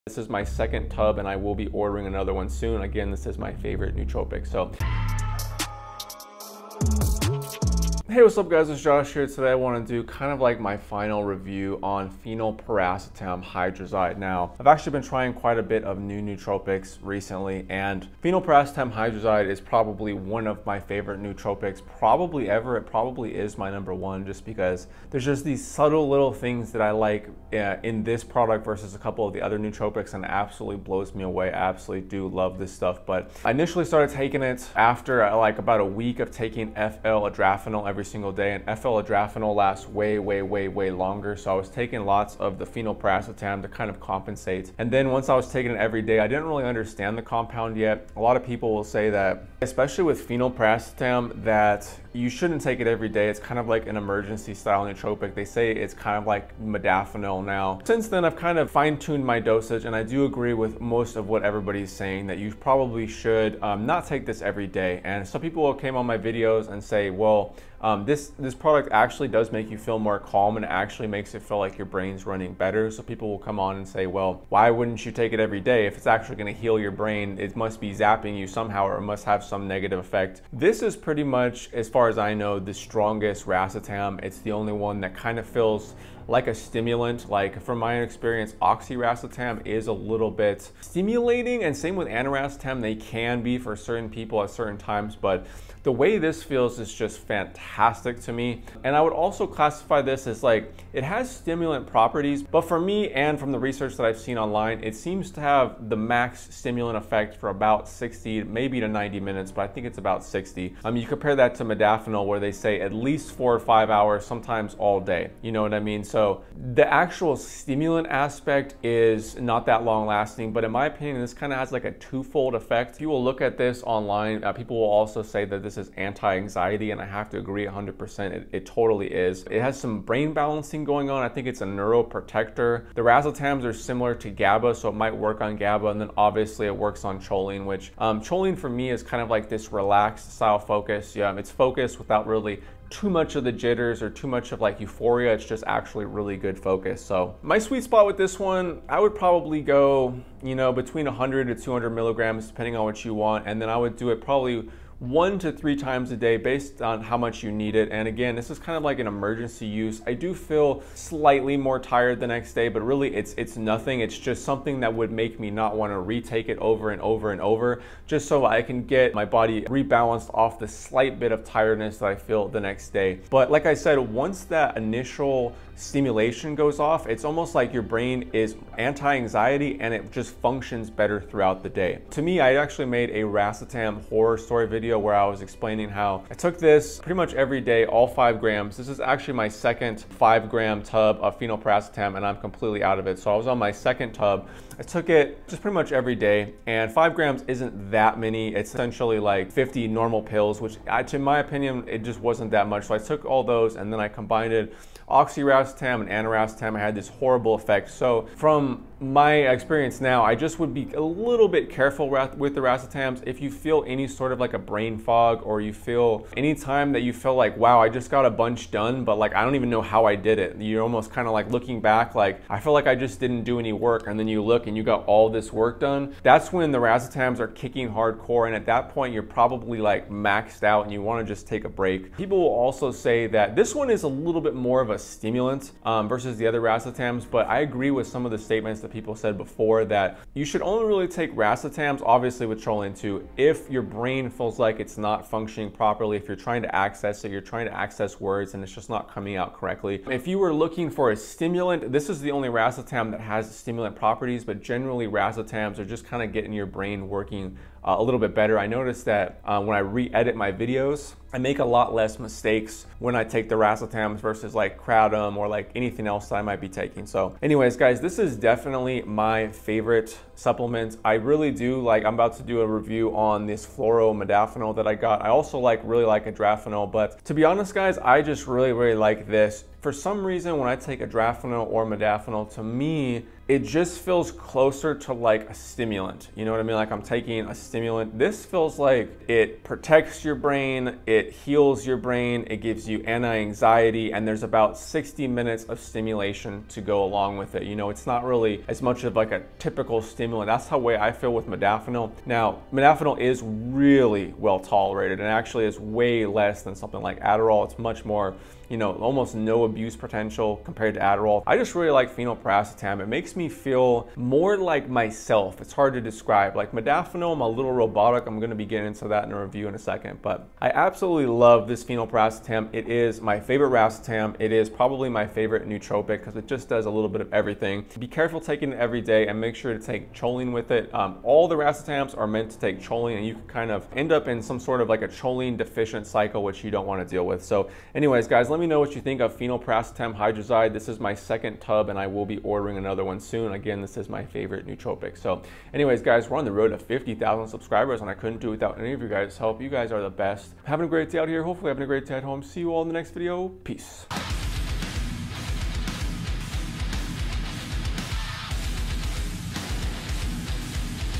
Phenyl Piracetam (2 Month Review) Hydrazide Version
Hello Friends! Today I wanted to share my thoughts on Phenyl Piracetam Hydrazyde, my favorite overall nootropic, have you tried phenyl piracetam hydrazide

Send Me Anything! Address Below 👇
RealJoshTv
United States